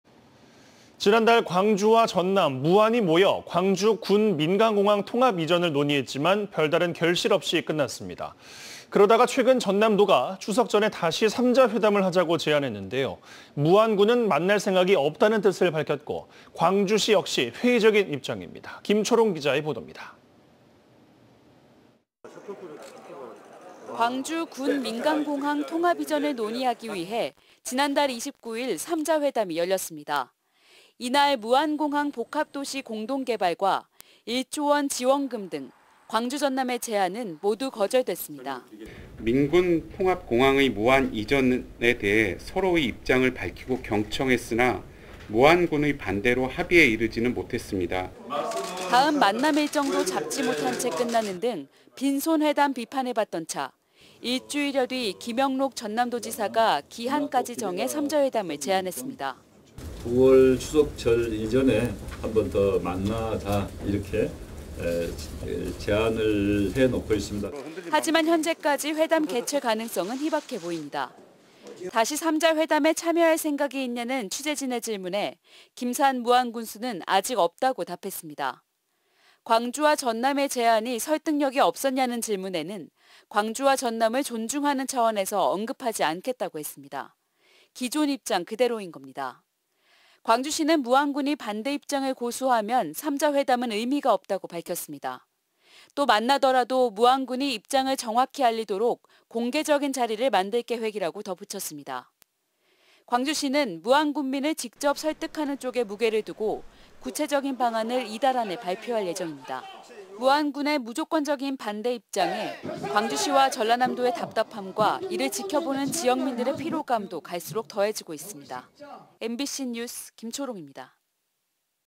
광주 군·민간공항 통합 이전 3자회담 제안에 무안군 “생각 없다” -R(240811일/뉴스데스크)
지난달 광주와 전남, 무안이 모여, 광주 군·민간공항 통합이전을 논의했지만,
별다른 결실 없이 끝났습니다.
 그러다가 최근 전남도가 추석 전에 다시 3자회담을 하자고 제안했는데요.

무안군은 만날 생각이 없다는 뜻을 밝혔고,
광주시 역시 회의적인 입장입니다.

김초롱 기자의 보도입니다.

◀ 리포트 ▶

광주 군·민간공항 통합이전을 논의하기 위해
지난달 29일 3자회담이 열렸습니다.

이날 무안공항 복합도시 공동 개발과
1조 원 지원금 등
광주·전남의 제안은 모두 거절됐습니다.

◀ SYNC ▶배일권 광주시 기획조정실장 (지난달 30일)
"민·군 통합공항의 무한 이전에 대해
서로의 입장을 밝히고 경청했으나,
무안군의 반대로 합의에 이르지는
못했습니다."

다음 만남 일정도 잡지 못한 채 끝나는 등
'빈손 회담' 비판을 받던 차,
일주일여 뒤 김영록 전남도지사가
기한까지 정해 3자회담을 제안했습니다.

◀ SYNC ▶김영록 전남도지사 (지난 6일)
“9월 추석절 이전에 한 번 더 만나자,
이렇게 제안을 해놓고 있습니다.”

하지만 현재까지
회담 개최 가능성은 희박해 보입니다.

(CG1)
다시 3자회담에 참여할 생각이 있냐는
취재진의 질문에
김산 무안군수는 “아직 없다”고 답했습니다.//

(CG2)
광주와 전남의 제안이
설득력이 없었냐는 질문에는
“광주와 전남을 존중하는 차원에서
언급하지 않겠다.”고 했습니다. //

기존 입장 그대로인 겁니다.

광주시는 무안군이 반대 입장을 고수하면,
3자회담은 의미가 없다고 밝혔습니다.

또 만나더라도,
무안군이 입장을 정확히 알리도록
공개적인 자리를 만들 계획이라고
덧붙였습니다.

광주시는 무안군민을
직접 설득하는 쪽에 무게를 두고,
구체적인 방안을 이달(8월) 안에
발표할 예정입니다.

무안군의 무조건적인 반대 입장에
광주시와 전라남도의 답답함과
이를 지켜보는 지역민들의 피로감도
갈수록 더해지고 있습니다.

MBC뉴스 김초롱입니다.